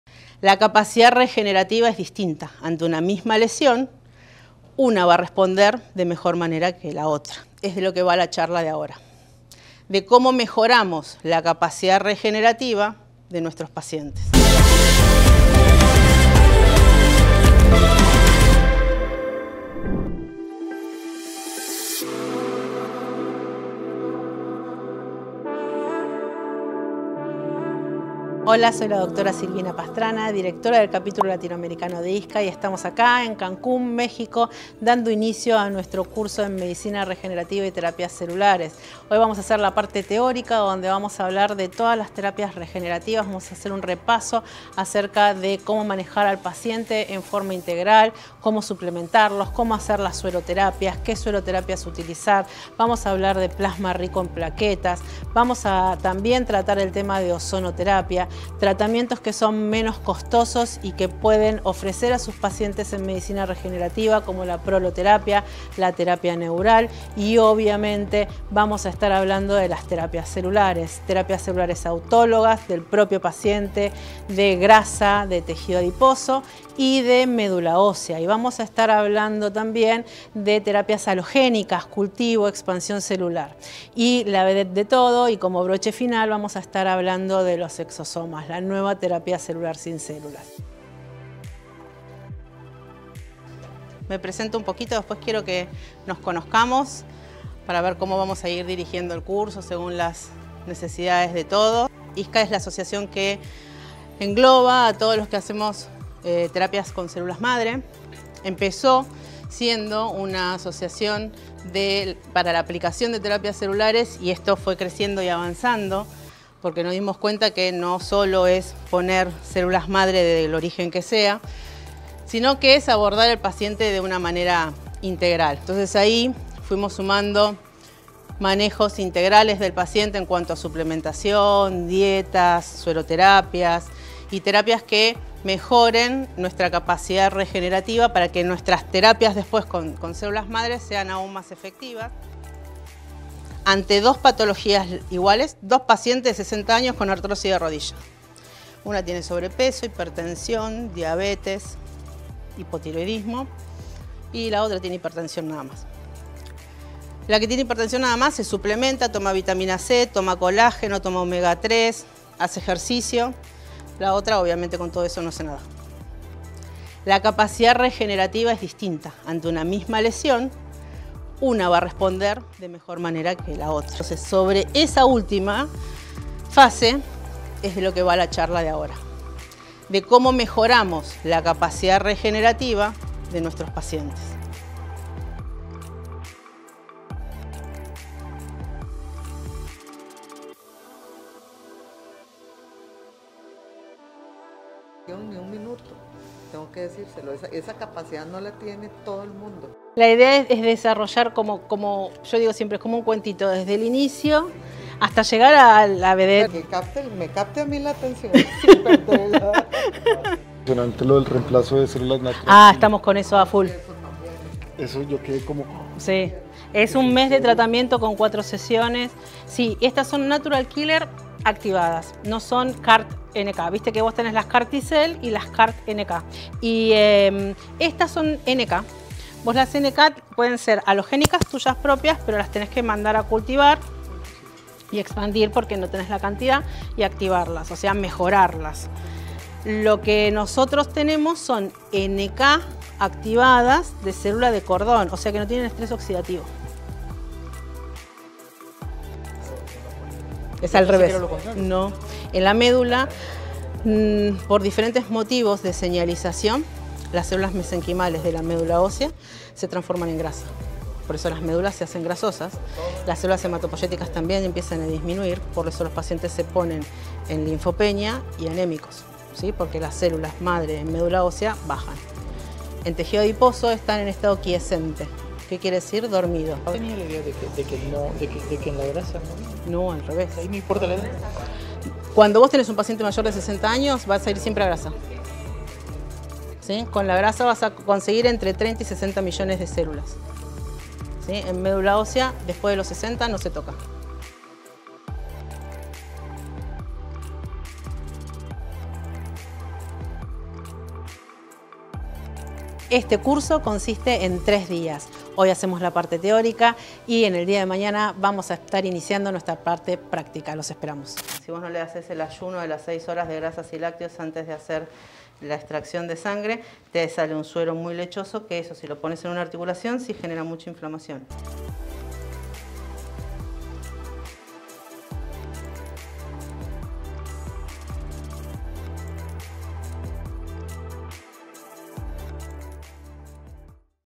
Vlog: Fundamentos Para Aplicar Medicina Regenerativa y Terapias Avanzadas | Cancún, México 🇲🇽
Durante la porción teórica de nuestro curso en Terapias Avanzadas en Cancún, México, la Dra. Silvina Pastrana discutió cómo mejorar la capacidad regenerativa, así como los fundamentos de la medicina regenerativa y las terapias avanzadas y cómo manejar a los pacientes de manera integral a través suplementos, dietas y sueroterapia.